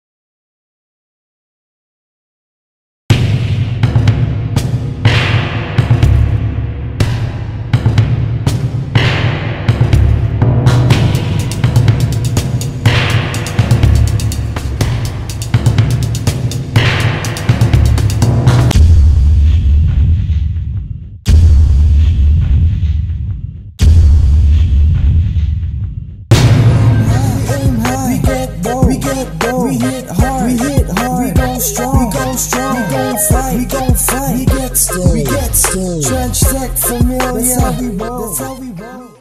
Available at the TJSHS Music Club booth at the 2016 Pika Fest (vendor #2)
SOME of the Trench Tech gear will be available on February 13th at the Tinian Jr./Sr. High School Music Club booth during the 12th annual Tinian Hot Pepper festival.

Thanks again brutha Cuki Alvarez and the whole Trench Tech fam for being a part of this year's fundraiser! SUPPORT ALL THINGS LOCAL! Shoo Shooots!

and please "like" all their pages on Facebook

Git At Me At:
kik @ ZosZengZung